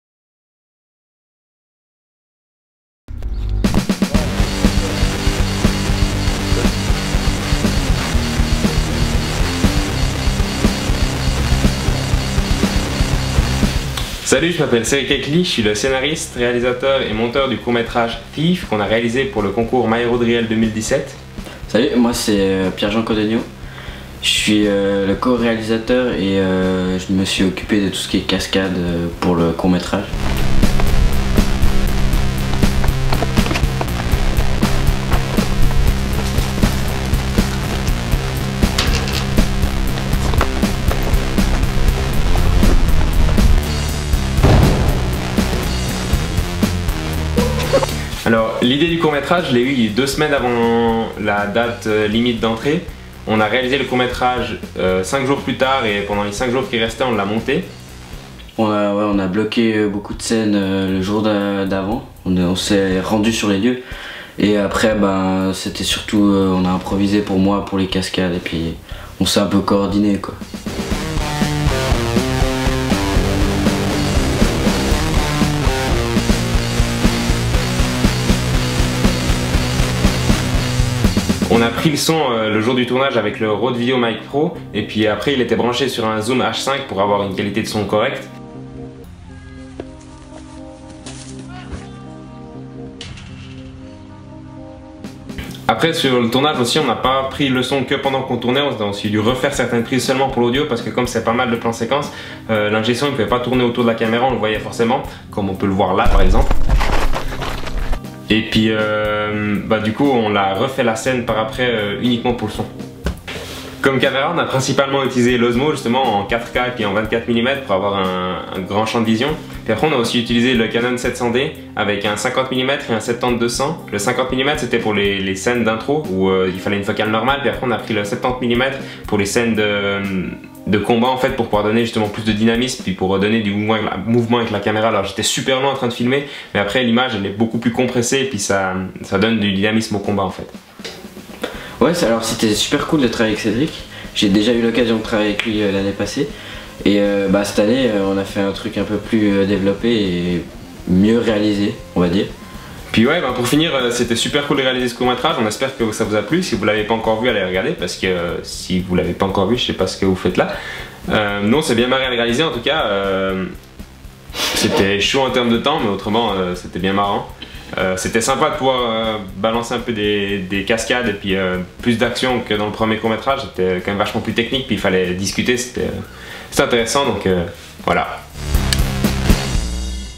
My Rode Reel - THIEF - Making of
Watch how we made the short film "THIEF".

Written - directed - cinematography and editing by Cédric Heckly
Co-directed and stunt supervisor : Pierre-Jean Codogno
Sound : Loic Sudan
Starring : Adrien Buchs - Pierre-Emannuel Codogno - Phillippe Rozumek - Pierre-Jean Codogno
Making of by Pierre-Jean Codogno